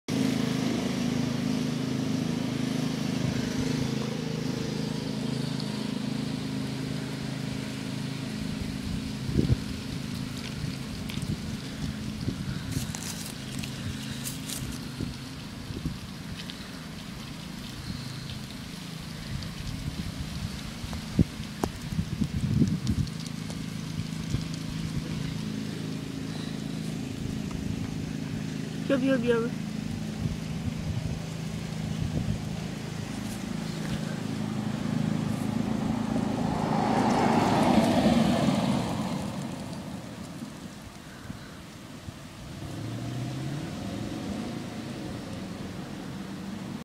Lawn mower
New news report: buy a G-PIG lawn mower and it"l have your lawn moved in no time! Sit it down on the lawn and it chomps away! No hard work!

Just kidding ( obviously ) don't use guinea pigs as lawn mowers. :p